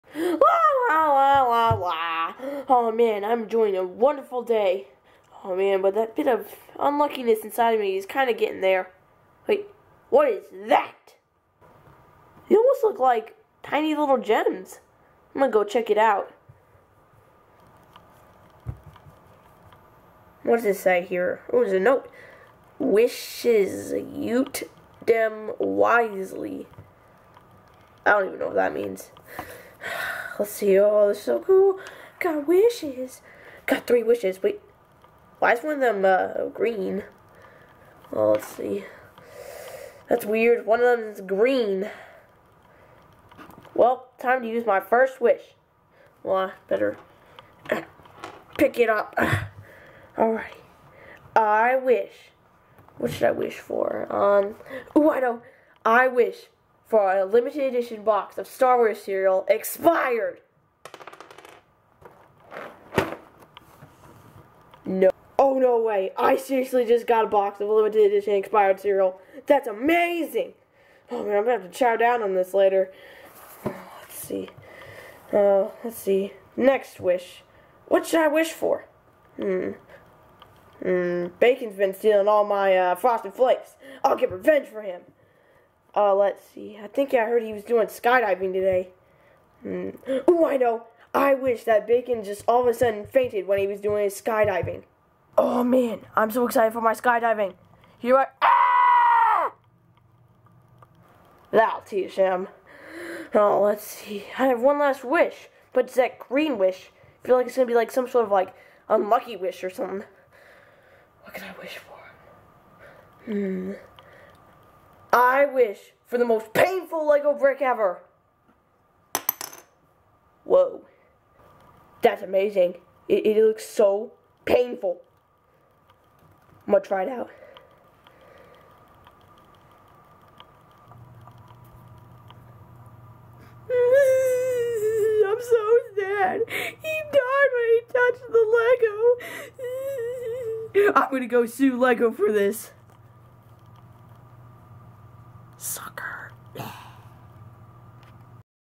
SLE Short: 3 Wishes!
Charley the Bear finds 3 wishes!